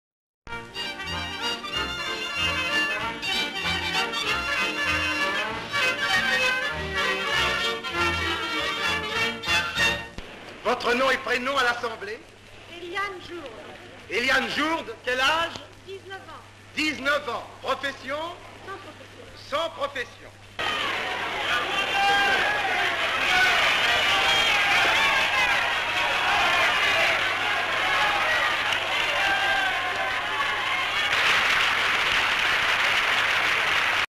"The Judgement Of Paris" - 1932! (1930-1931)
Full title reads: "'The judgement of Paris' - 1932! Uproar greets Jury's hotly disputed election of 19 years old blond Mlle. Elaine Jourde, beauty Queen of Paris for 1932."

Paris, France.

An audience watch the proceedings for the judging of the beauty contest. We see Mlle Jourde on stage. Much shouting from the crowd as the finalists line up.

CU the winner - Elaine Jourde.

N.B. Film quality is not very good.
 FILM ID:655.27